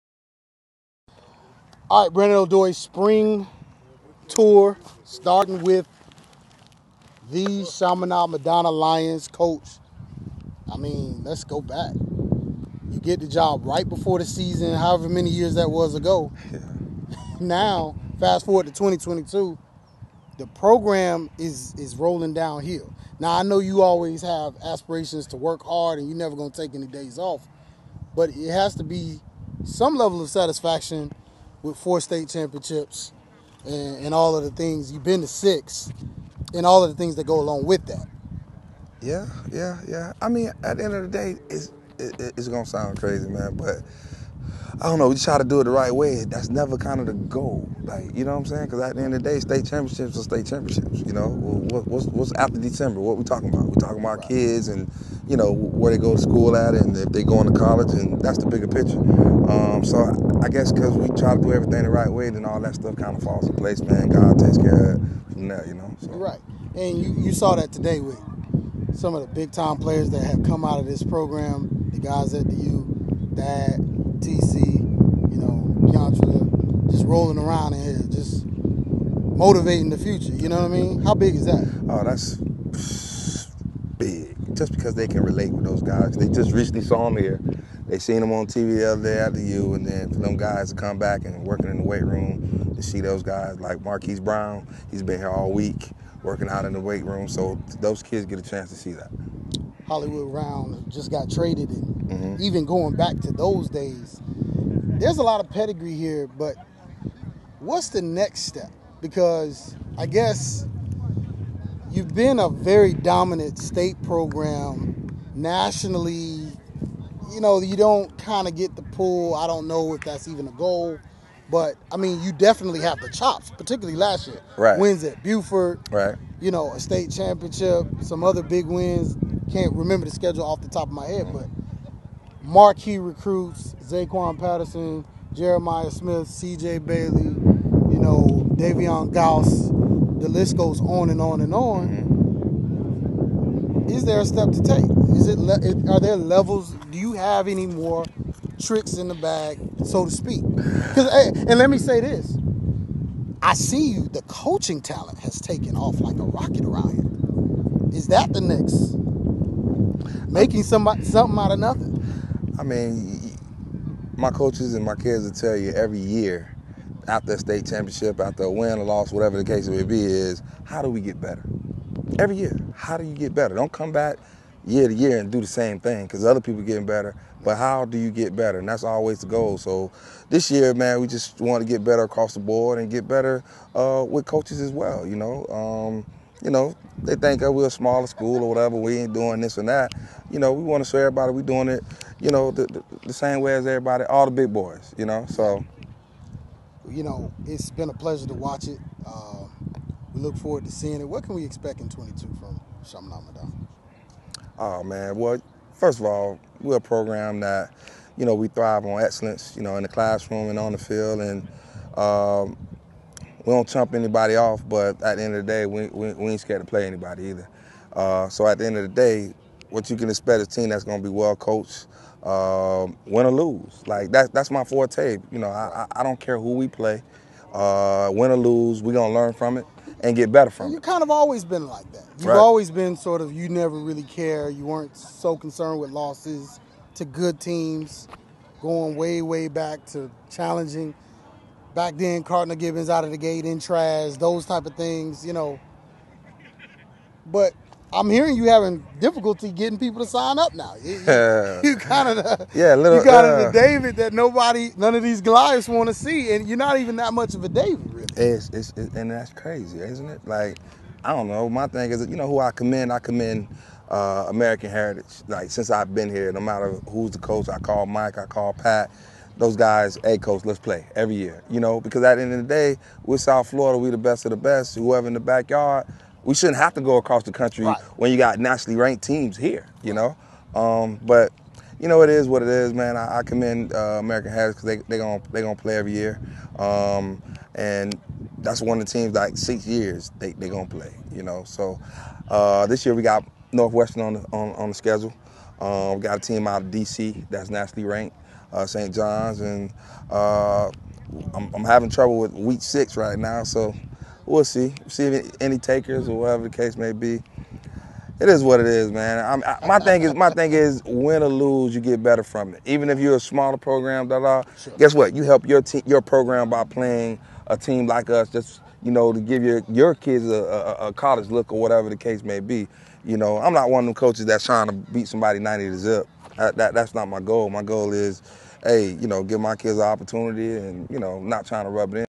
One on One: Dameon Jones, head coach Chaminade-Madonna (Hollywood, FL)
Four time Florida 3A state championship coach speaks to Brandon Odoi of Football Hotbed